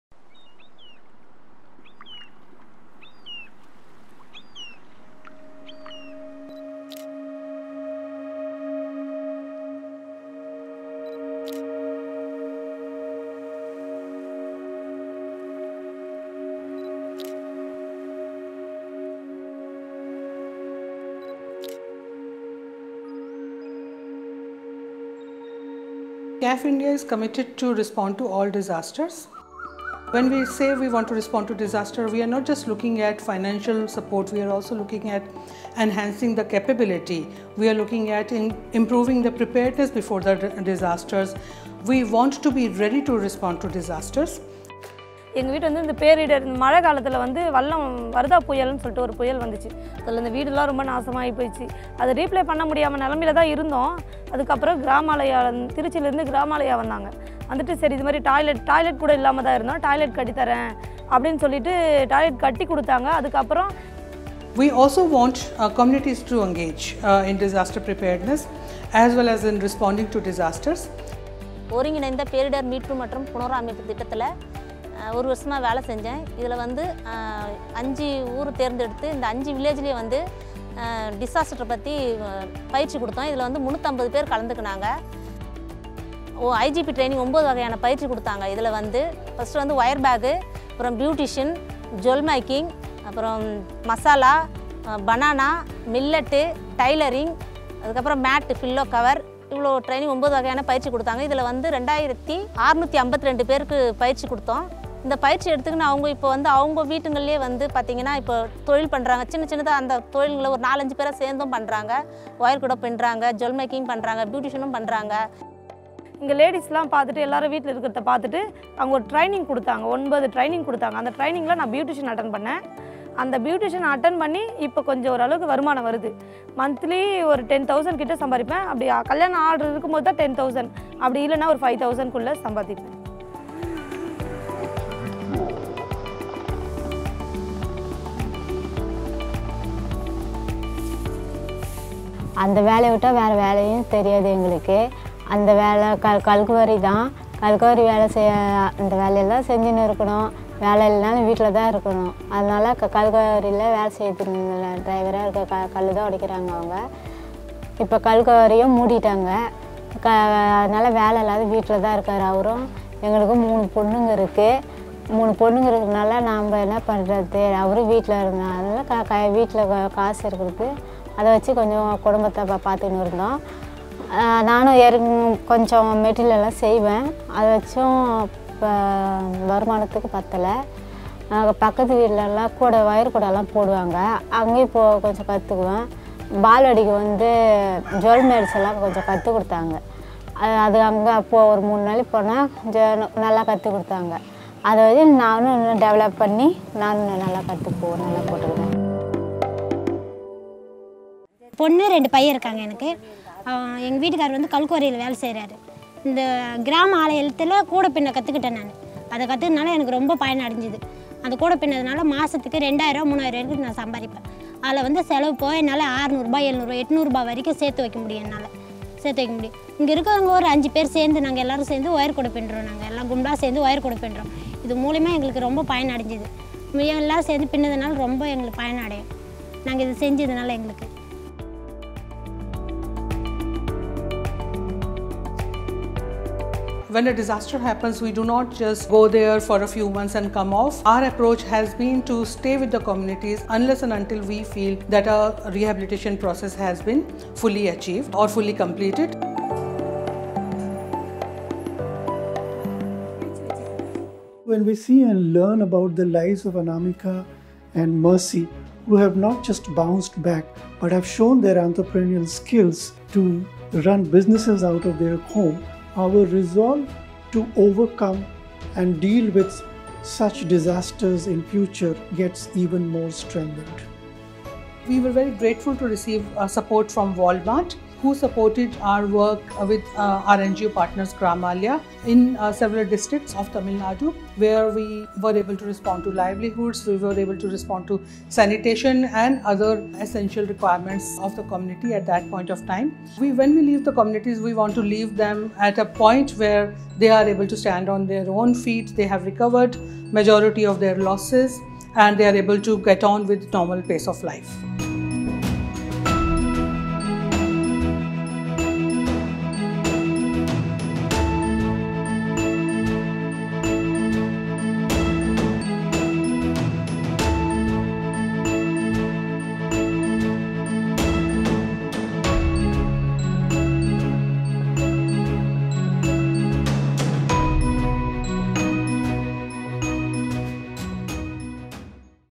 Charities Aid Foundation: Project Impact Video
An impact video on CAF India's Post Disaster Rehabilitation and Recovery Program showcasing the empowered communities through inclusive growth opportunities.

Video by Mosaic Media